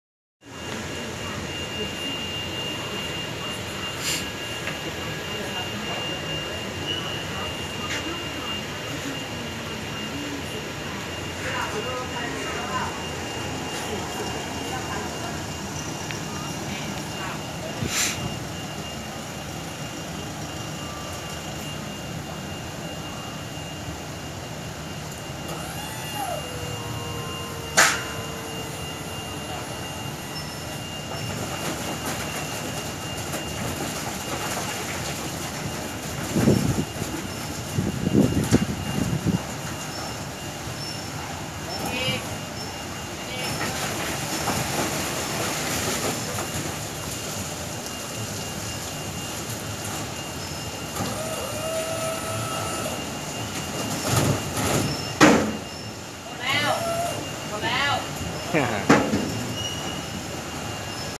การใช้งานกระบะบรรทุก Toyota Revo ดั๊มพ์
การใช้งานกระบะแสตนเลส Toyota Revo ดั๊มพ์เล็ก 2 ตัน Shinmaywa@japan ชุดควบคุมแรงดันไฮดรอลิค Power pack เมอร์เตอร์ไฟฟ้า 12 โวลล์ อิตาลี @ Italy